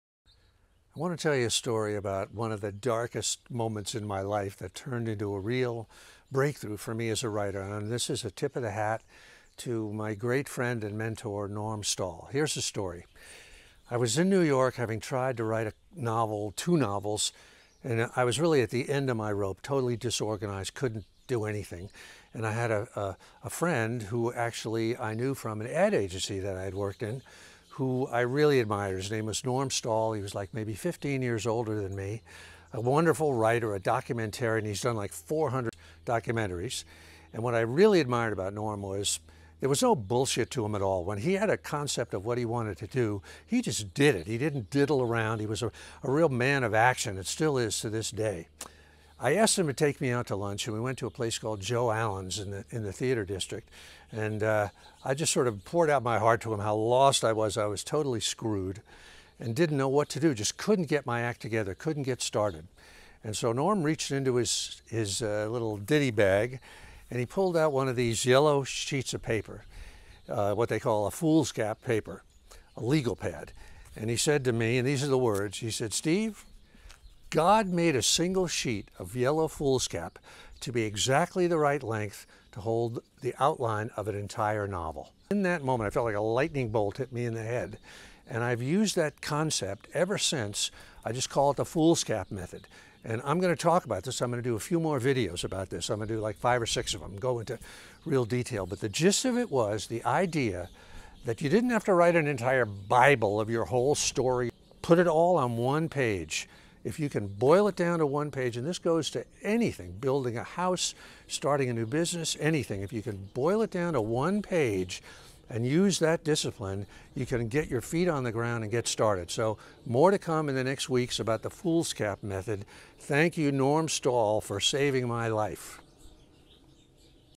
Foolscap Method Introduction
If you following Writing Wednesdays you’ve probably read about something I call The Foolscap Method. I give credit to this to my great friend and mentor Norm Stahl.

I was in NYC, at a really low point, couldn’t get anything going, and I went to lunch with Norm, a no nonsense- call it as he sees it guy. I told him my troubles and he pulled out a yellow legal pad – a foolscap paper, as it is sometimes called. He said “Steve, God made a single sheet of yellow foolscap to be exactly the right length to hold the outline of an entire novel.”

It was like I was struck by lightning. What? No need for a Bible length outline? I can put I all on one page? Well, it worked and it’s the way I’ve worked ever since. I call it The Foolscap Method.

It’s not just for writing–if you’re building a house, planning a business–get it onto one page of foolscap. Sometimes the outlining of something can be so overwhelming that you stop before you even get started.

I’m going to do a few more videos about this. I’ll also put a link to in my bio to some things I’ve written in the blog about The Foolscap Method.

Thanks to Sys Trier Morch for the pictures of Norm. @systm

And thanks to pirate republic brewery for the shirt!